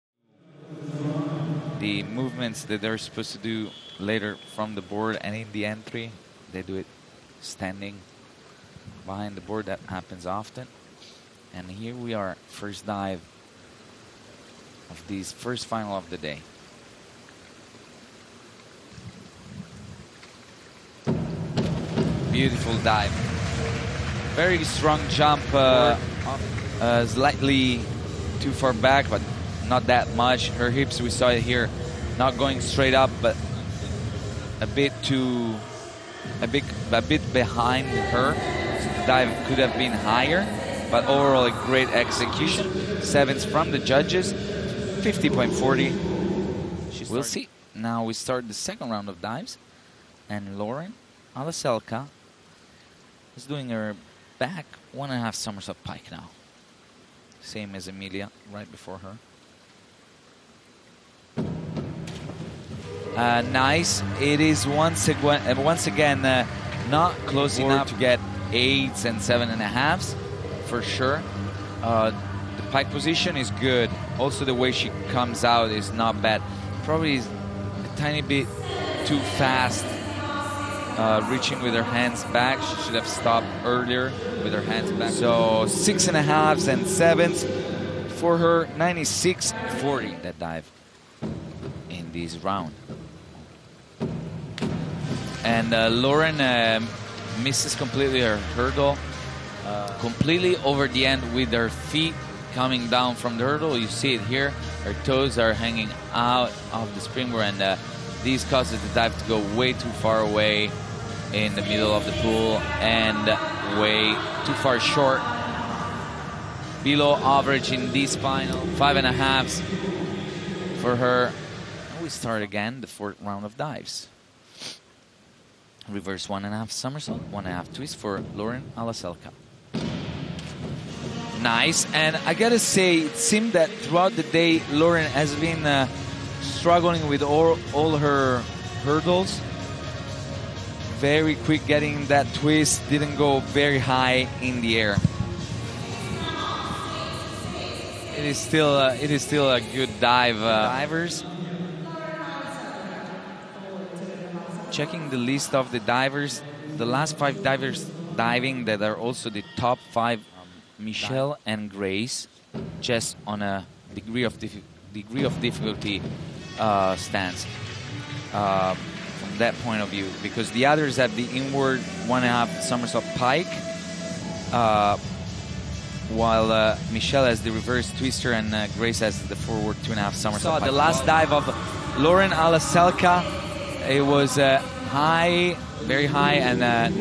Lauren HALLASELKA - 1m Springboard Divig Final Highlights
Lauren HALLASELKA's performance is nothing short of spectacular, showcasing the dedication and talent that makes her a standout in the world of diving.  Join us in celebrating this extraordinary athlete's achievements and relive the excitement of each dive in this exclusive highlight reel.